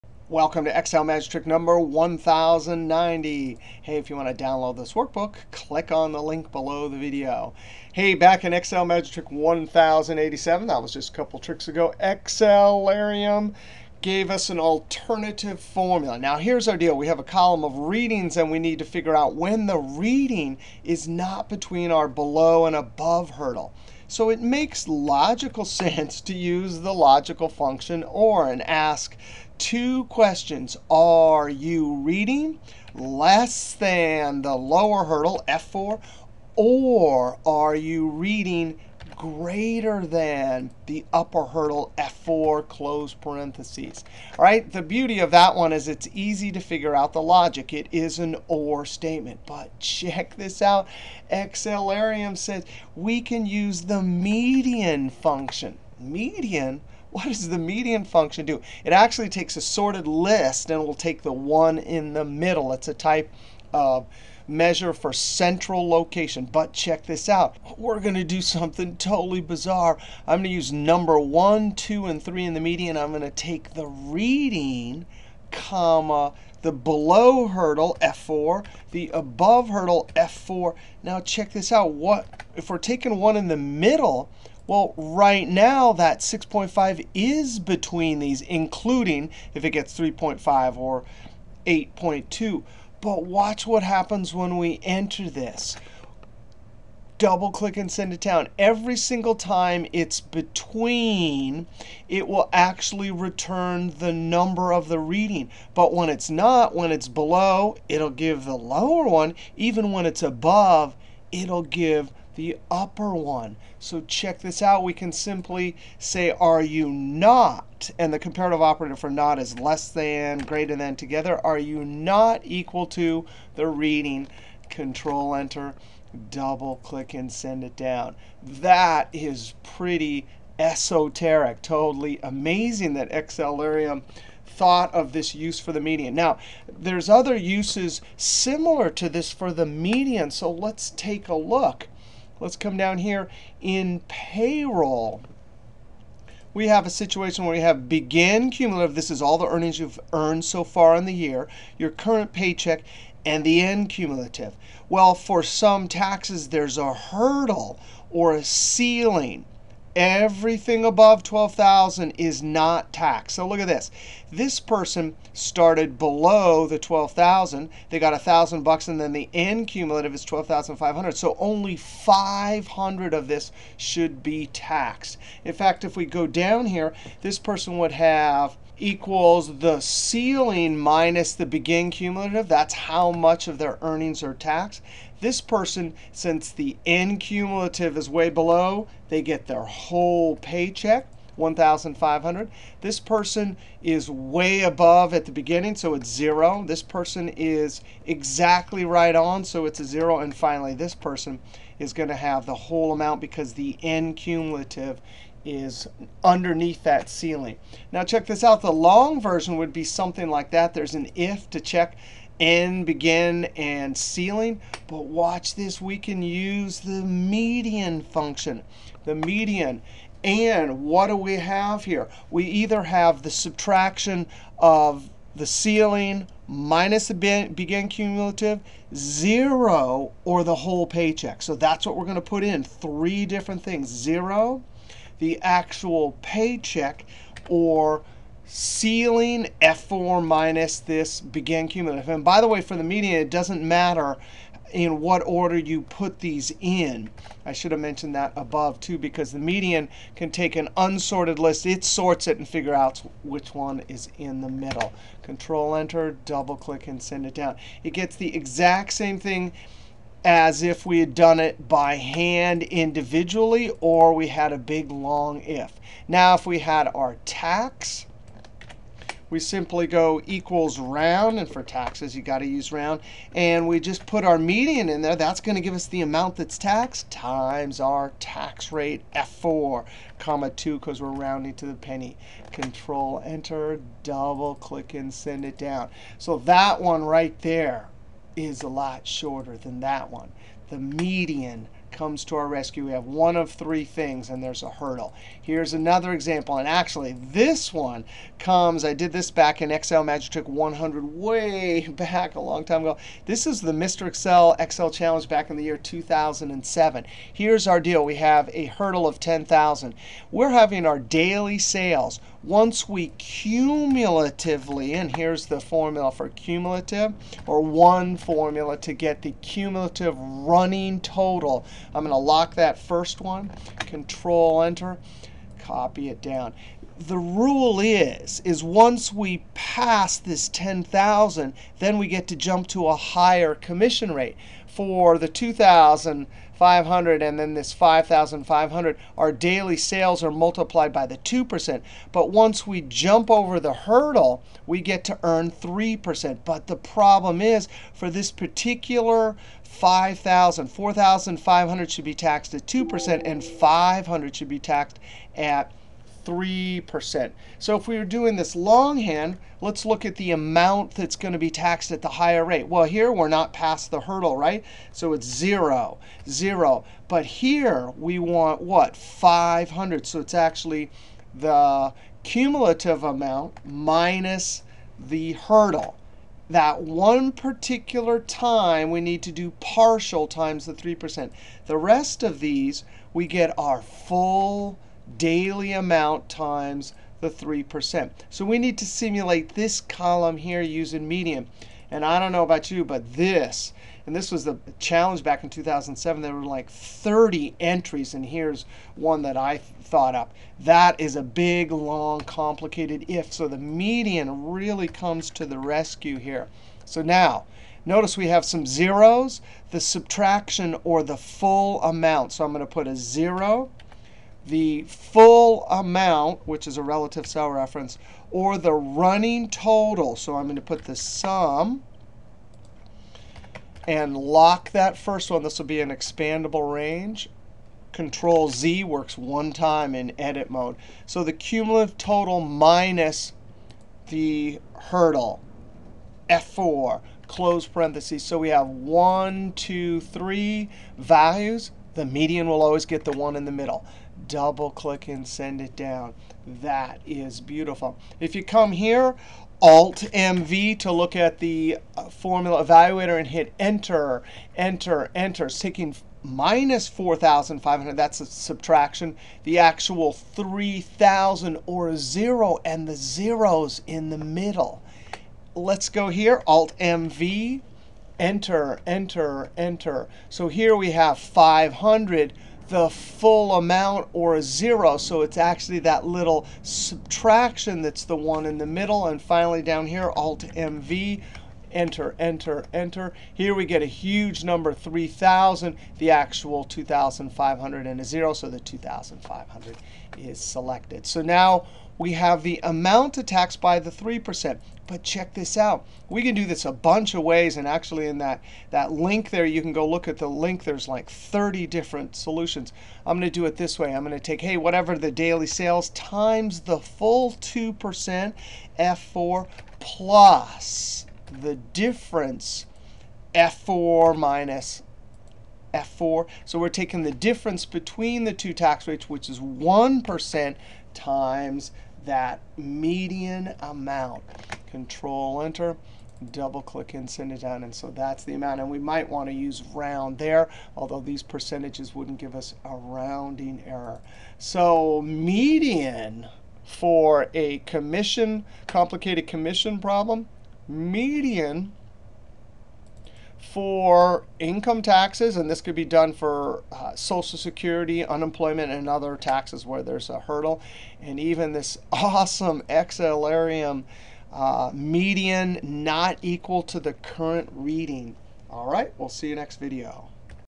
Excel Magic Trick 1090: MEDIAN Function 3 Amazing Uses: OR Criteria, Payroll, Commissions
See how to use the MEDIAN function is unusual ways that allow you to shorten the formula (for last two examples):
1. (00:09 min) Deliver an OR Criteria Logical Value. (Cool trick from Xlarium from YouTube.)
2. (02:15 min) Payroll Tax calculation where there is a hurdle, past which no tax is paid. (Ceiling Amount). By using MEDIAN we avoid complicated IF function for payroll calculation. Use MEDIAN to pick out middle value from three values: 0, full pay check amount or partial paycheck amount (Ceiling for Tax minus Begin Cumulative Amount).
3. (04:51 min) Commission calculation where there is a hurdle Amount, past which you earn a higher commission rate. Avoid more complicated formula by using MEDIAN to pick out middle value from three values: 0, Running Total minus Hurdle for Higher Commission Rate.

Cool formula tricks with MEDIAN function